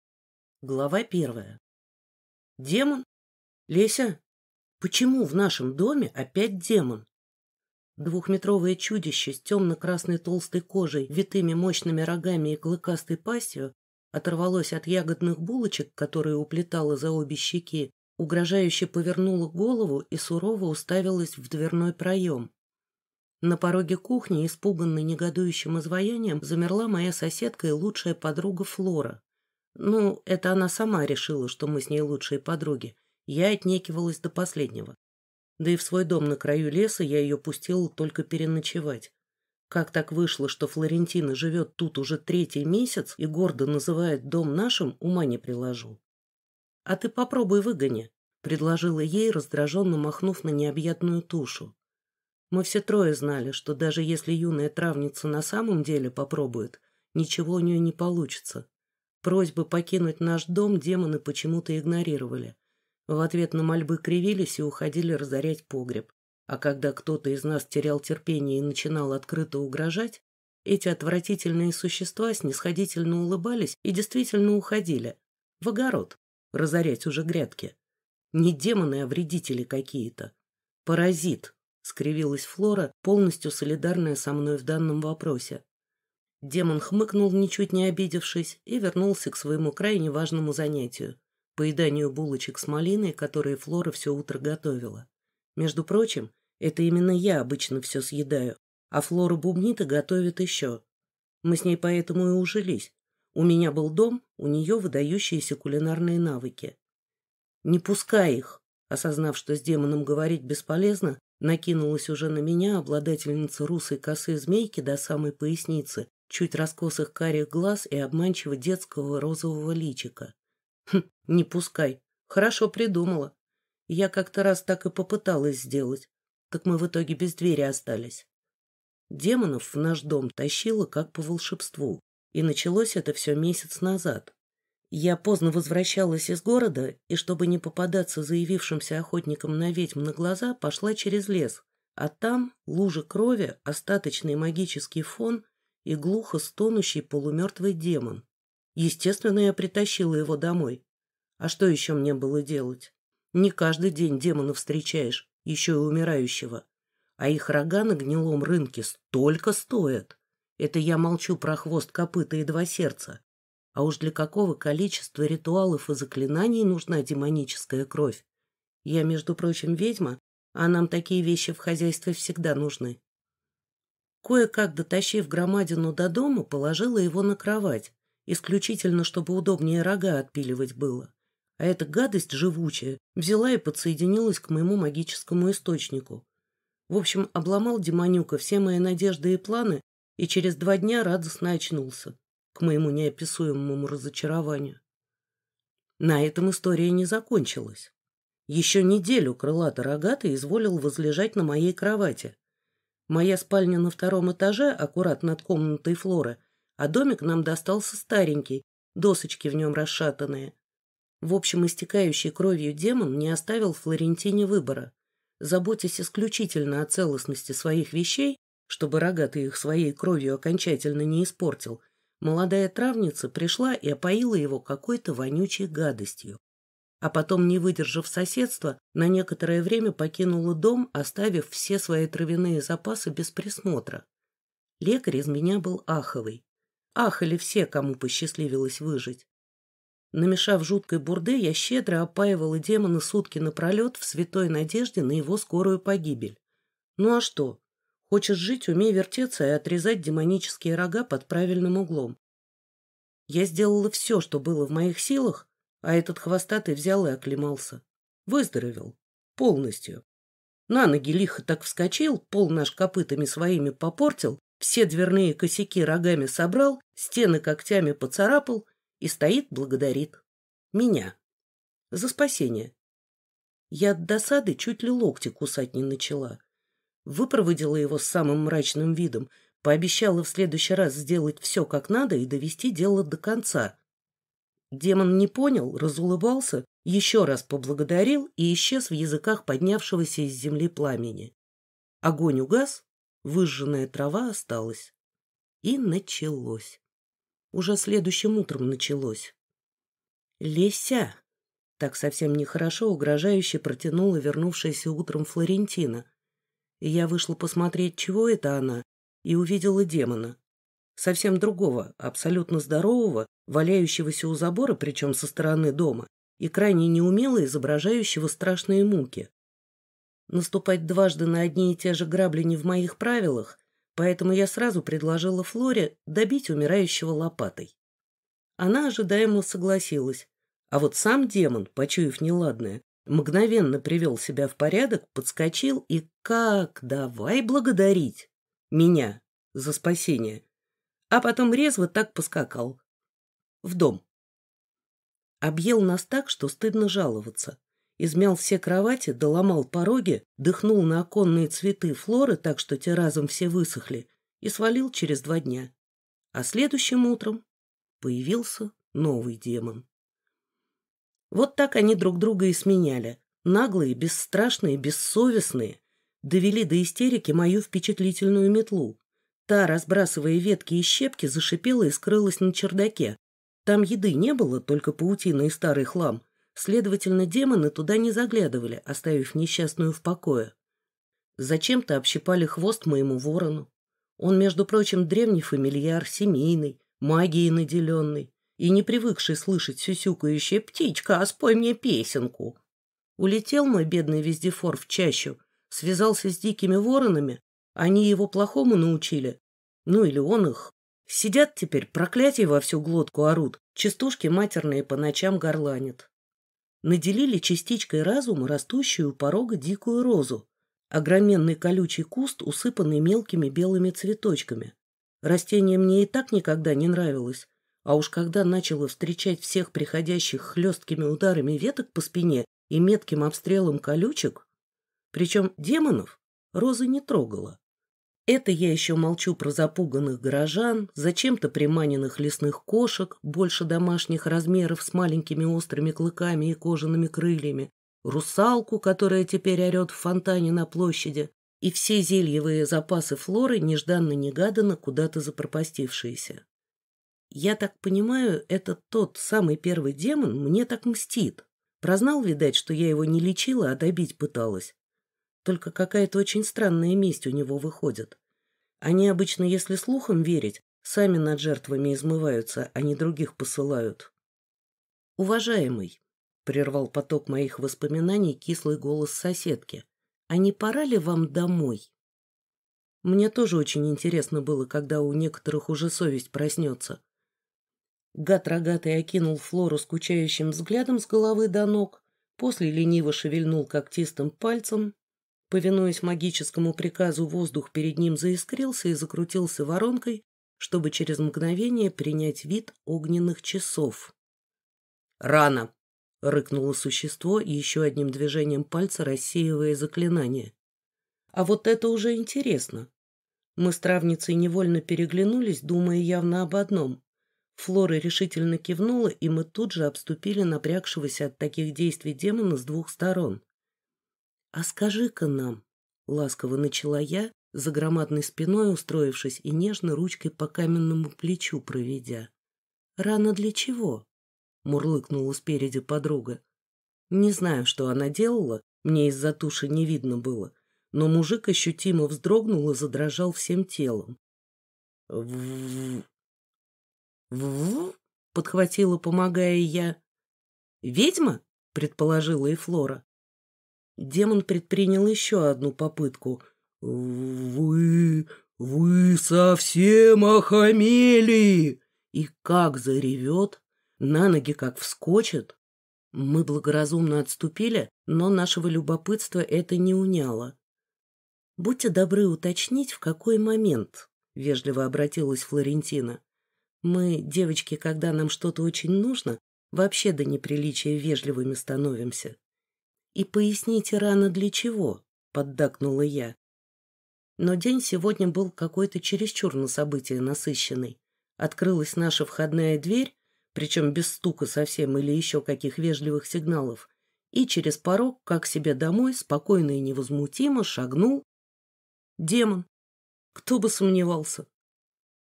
Валентина Гордова – Украсть ведьму. [Аудиокнига]
Аудиокнига Украсть ведьму Валентины Гордовой – слушать онлайн.
Автор: Валентина Гордова.
Жанр: юмористическое фэнтези
Дата выхода на ЛитРес: 29 августа 2023
Дата написания: 2022
Чтец: Нина Медникова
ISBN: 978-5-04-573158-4

Описание: Я лишь пыталась добить и без того умирающего демона. А что такого? Он бы всё равно умер, а так у меня была возможность озолотиться, распродав его тело по кусочкам на Гнилом рынке. Но вместо того, чтобы послушно скончаться, рогатый подсоединился к моему магическому источнику и… выздоровел. Полностью. И началось! Вот с нашествия бессовестных бесов на мой дом всё и началось!